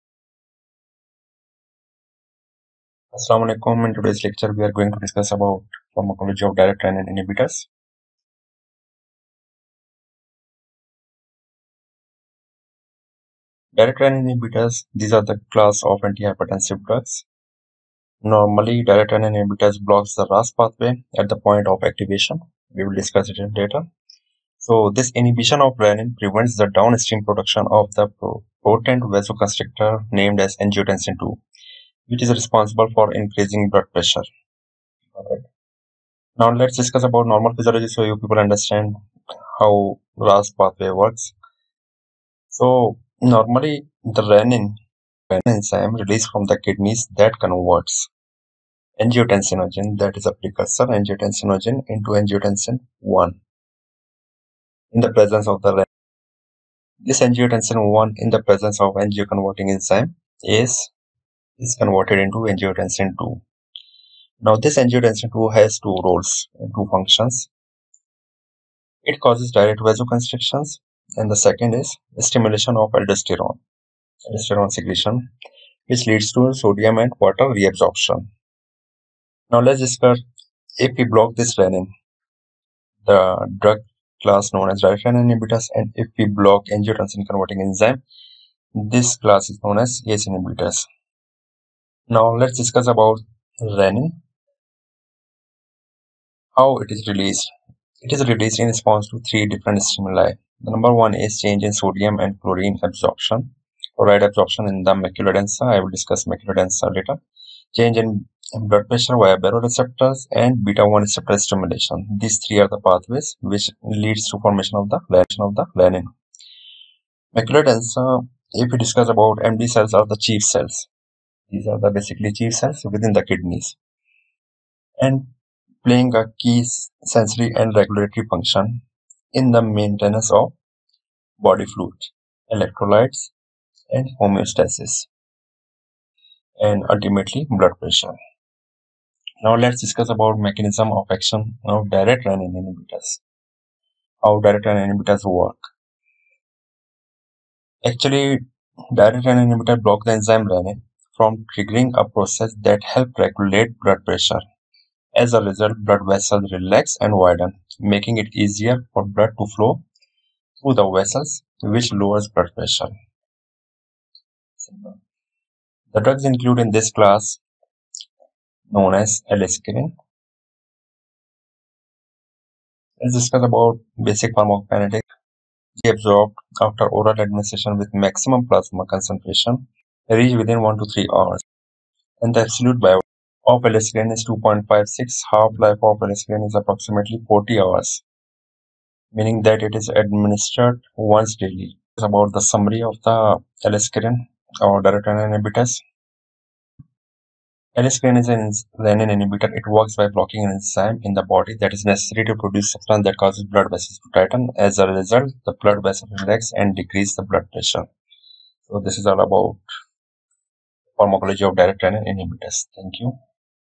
Direct Renin Inhibitors - Aliskiren
Renin inhibitors pharmacology and mechanism of action. Renin inhibitors are pharmaceutical drugs inhibiting the activity of renin that is responsible for hydrolyzing angiotensinogen to angiotensin I, which in turn reduces the formation of angiotensin II that facilitates blood pressure.

direct renin inhibitors pharmacology,
direct renin inhibitors pixorize,
direct renin inhibitors simple nursing,
aliskren,
pharmacology of aliskren,
renin angiotensin system,